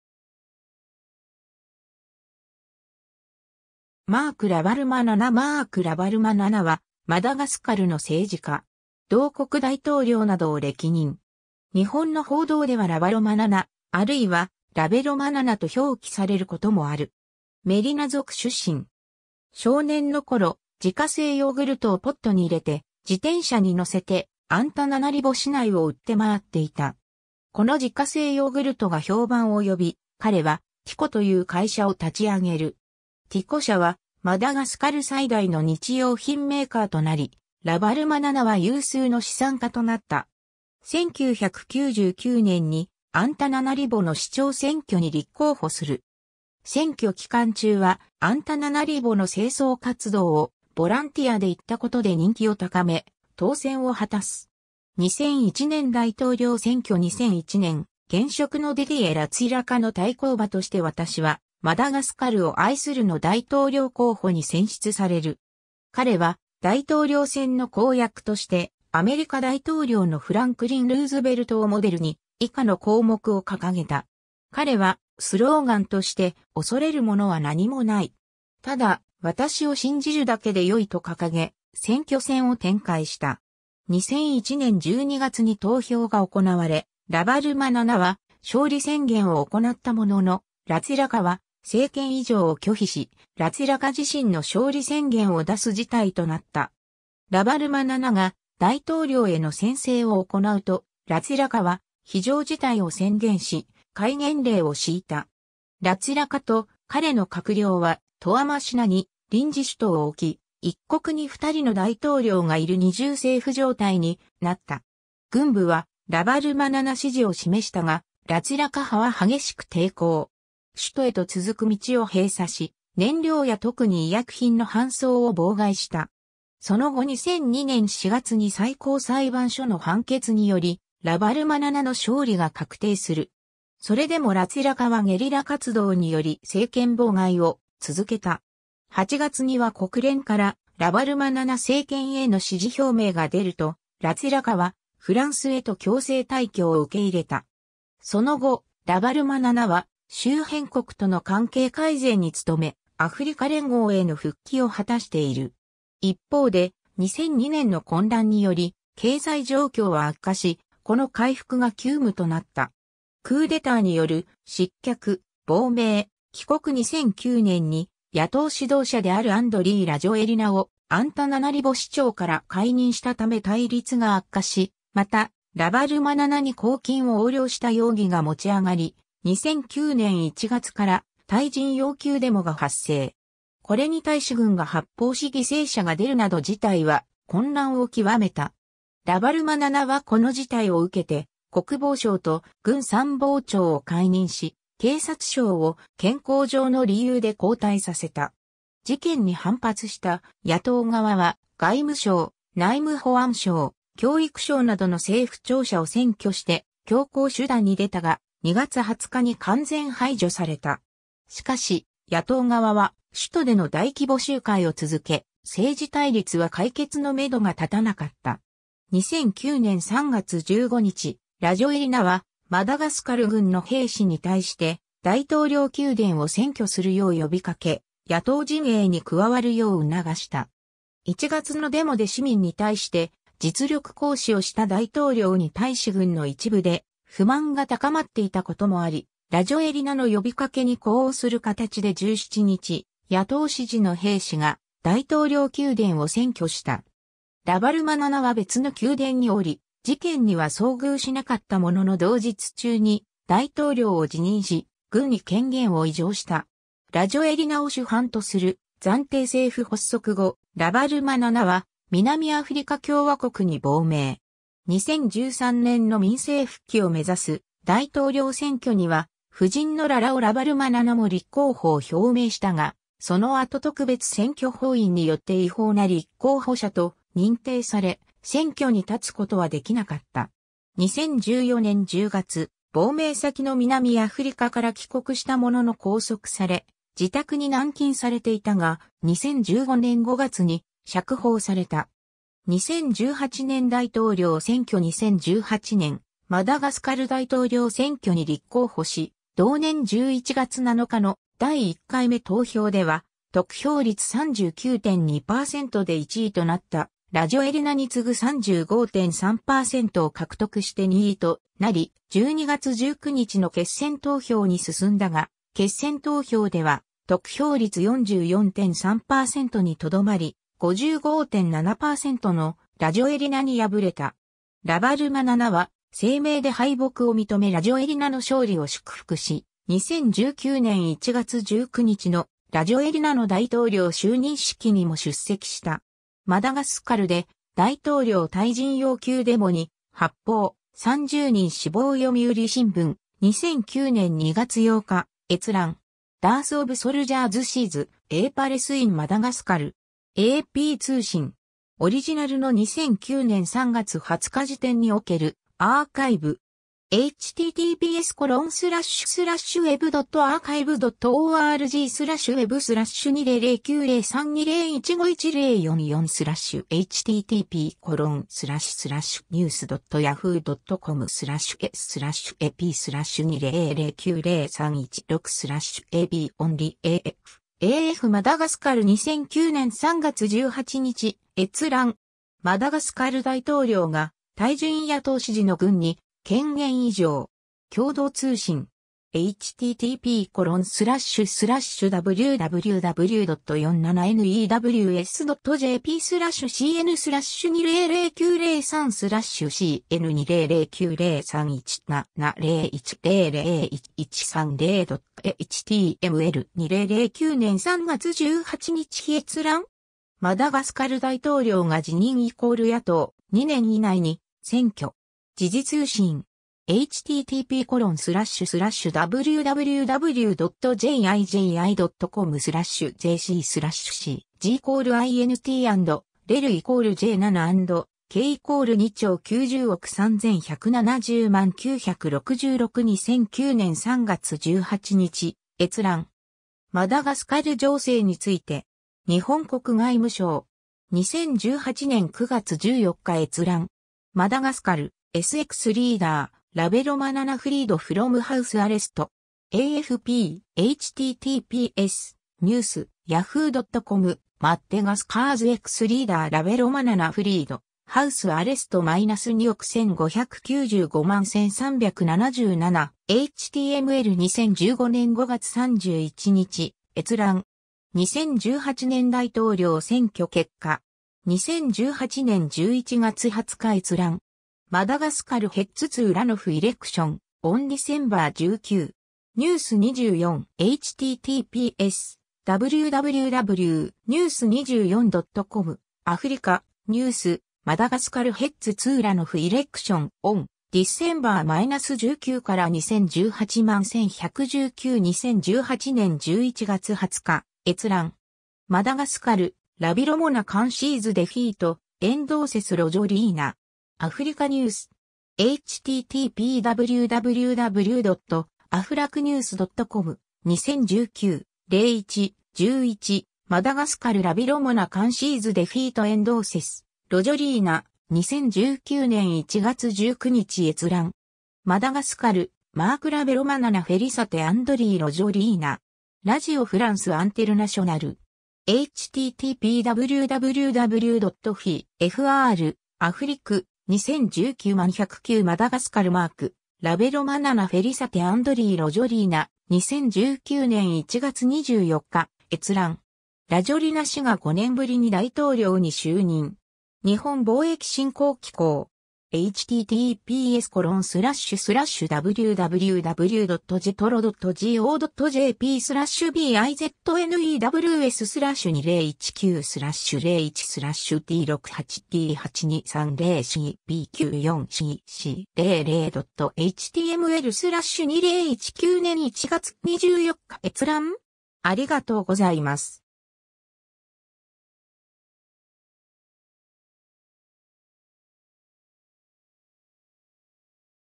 マーク・ラヴァルマナナ
マーク・ラヴァルマナナ マーク・ラヴァルマナナ（Marc Ravalomanana, 1949年12月12日 - ）は、マダガスカルの政治家。
同国大統領などを歴任。
日本の報道ではラバロマナナ、あるいはラベロマナナと表記されることもある。
 メリナ族出身。
少年のころ、自家製ヨーグルトをポットに入れて、自転車に乗せてアンタナナリボ市内を売ってまわっていた。
この自家製ヨーグルトが評判を呼び、彼はティコという会社を立ち上げる。
ティコ社はマダガスカル最大の日用品メーカーとなり、ラヴァルマナナは有数の資産家となった。
 1999年に、アンタナナリボの市長選挙に立候補する。
選挙期間中はアンタナナリボの清掃活動をボランティアで行ったことで人気を高め、当選を果たす。
 2001年大統領選挙 2001年、現職のディディエ・ラツィラカの対抗馬として私はマダガスカルを愛する（通称：TIM党）の大統領候補に選出される。
彼は大統領選の公約として、アメリカ大統領のフランクリン・ルーズベルトをモデルに、以下の項目を掲げた。
 彼は、スローガンとして「恐れるものは何もない。ただ私を信じるだけでよい」と掲げ、選挙戦を展開した。
 2001年12月に投票が行われ、ラヴァルマナナは勝利宣言を行ったものの、ラツィラカは政権移譲を拒否し、ラツィラカ自身の勝利宣言を出す事態となった。
ラヴァルマナナが大統領への宣誓を行うと、ラツィラカは非常事態を宣言し、戒厳令を敷いた。
ラツィラカと彼の閣僚は、トアマシナに臨時首都を置き、一国に2人の大統領がいる二重政府状態になった。
軍部はラヴァルマナナ支持を示したが、ラツィラカ派は激しく抵抗。
首都へと続く道を閉鎖し、燃料や特に医薬品の搬送を妨害した。
その後2002年4月に最高裁判所の判決により、ラヴァルマナナの勝利が確定する。
それでもラツィラカはゲリラ活動により政権妨害を続けた。
8月には国連からラヴァルマナナ政権への支持表明がでると、ラツィラカはフランスへと強制退去を受け入れた。
その後、ラヴァルマナナは、周辺国との関係改善に努め、アフリカ連合への復帰を果たしている。
一方で、2002年の混乱により経済状況は悪化し、この回復が急務となった。
 クーデターによる失脚、亡命、帰国 2009年に野党指導者であるアンドリー・ラジョエリナをアンタナナリボ市長から解任したため対立が悪化し、またラヴァルマナナに公金を横領した容疑が持ち上がり、2009年1月から退陣要求デモが発生。
これに対し軍が発砲し犠牲者が出るなど事態は混乱を極めた。
ラヴァルマナナはこの事態を受けて、国防相と軍参謀長を解任し、警察相を「健康上の理由」で交代させた。
事件に反発した野党側は、外務省、内務保安省、教育省などの政府庁舎を占拠して強硬手段にでたが、2月20日に完全排除された。
しかし、野党側は首都での大規模集会を続け、政治対立は解決の目処が立たなかった。
 2009年3月15日、ラジョエリナはマダガスカル軍の兵士に対して大統領宮殿を占拠するよう呼びかけ、野党陣営に加わるよう促した。
1月のデモで市民に対して実力行使をした大統領に対し軍の一部で不満が高まっていたこともあり、ラジョエリナの呼びかけに呼応する形で17日、野党支持の兵士が大統領宮殿を占拠した。
ラヴァルマナナは別の宮殿におり、事件には遭遇しなかったものの同日中に大統領を辞任し、軍に権限を委譲した（マダガスカル・クーデター）。
 ラジョエリナを首班とする暫定政府発足後、ラヴァルマナナは南アフリカ共和国に亡命。
2013年の民政復帰を目指す大統領選挙には夫人のララオ・ラヴァルマナナも立候補を表明したが、その後特別選挙法院によって違法な立候補者と認定され、選挙に立つことはできなかった。
 2014年10月、亡命先の南アフリカから帰国したものの拘束され自宅に軟禁されていたが、2015年5月に釈放された。
 2018年大統領選挙 2018年マダガスカル大統領選挙に立候補し、同年11月7日の第1回目投票では得票率39.2%で1位となったラジョエリナに次ぐ35.3%を獲得して2位となり、12月19日の決選投票に進んだが、決選投票では得票率44.3％にとどまり、55.7％のラジョエリナに破れた。
ラヴァルマナナは声明で敗北を認めラジョエリナの勝利を祝福し、2019年1月19日のラジョエリナの大統領就任式にも出席した。
 ^ マダガスカルで大統領退陣要求デモに発砲、30人死亡読売新聞、20...